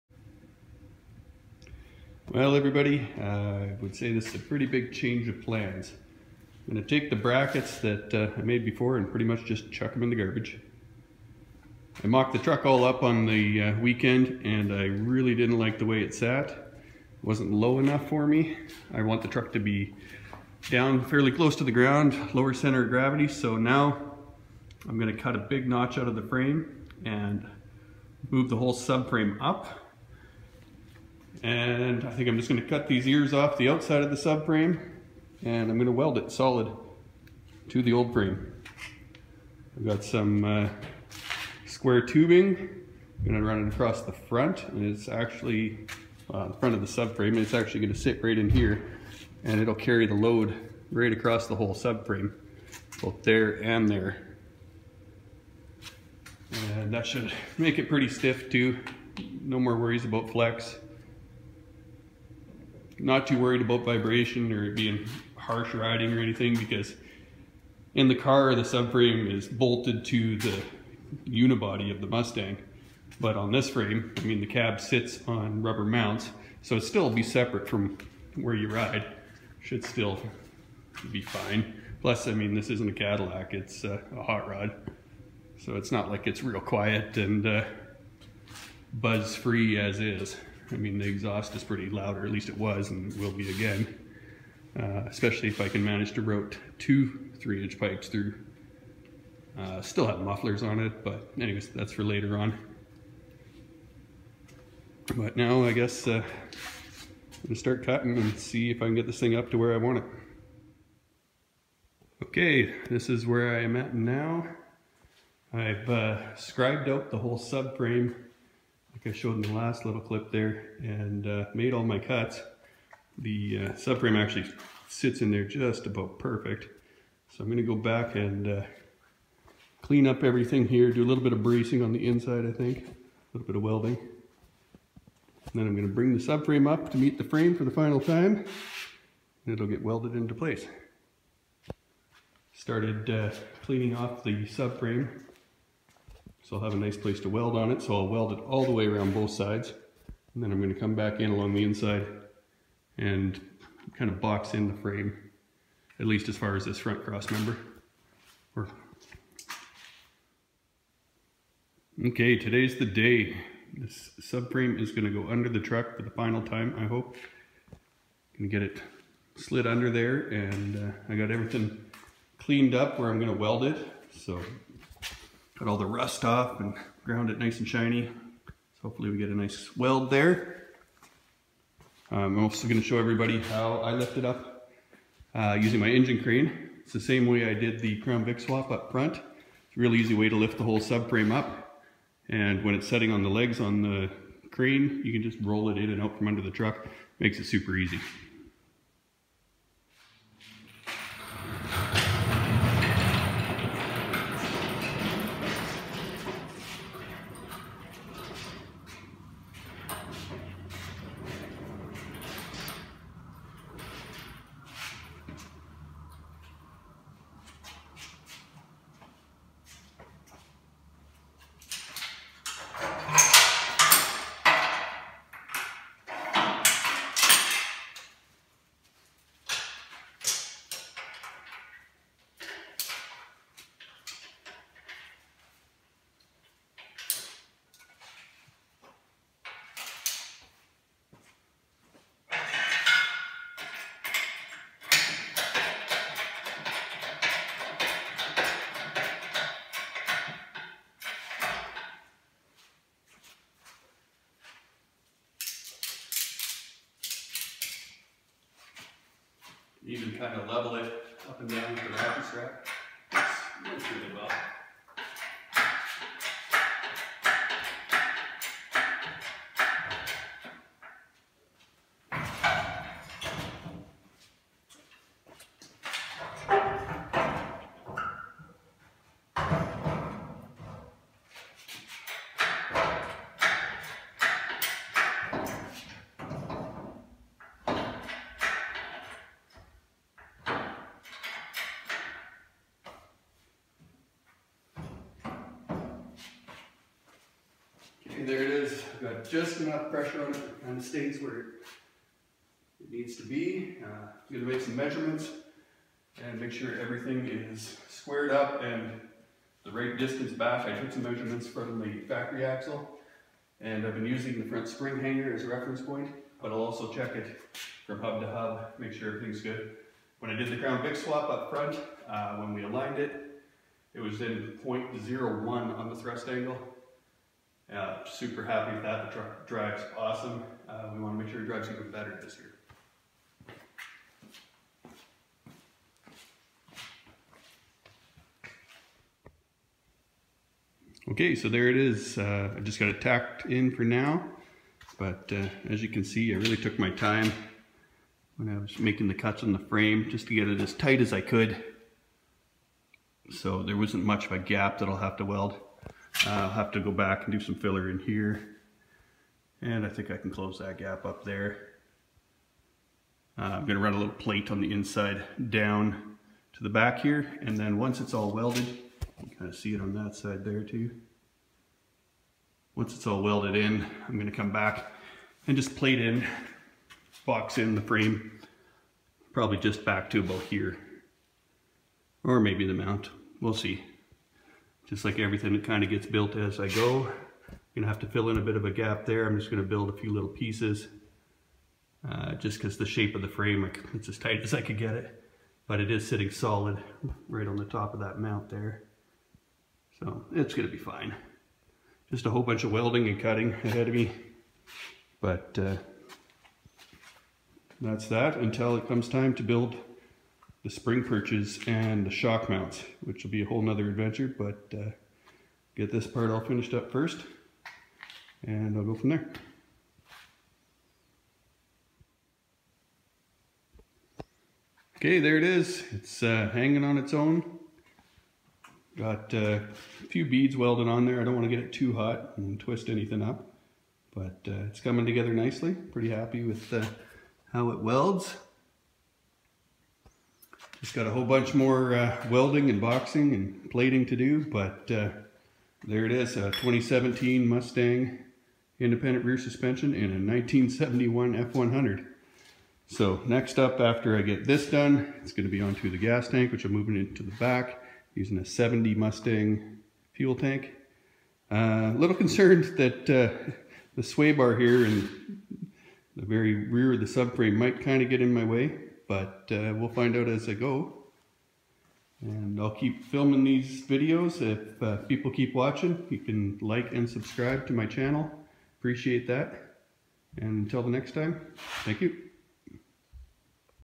1971 F100 gets 2017 Mustang IRS part 4
1971 F100 gets 2017 Mustang IRS part 4. In this episode I discover how much work this install will be. I didn't like the ride height at all with the subframe under the F100 frame. So I cut the frame! This lets me move the Mustang subframe up 3" to get the ride height I want.

If you'd like to help out with parts and future builds you can contribute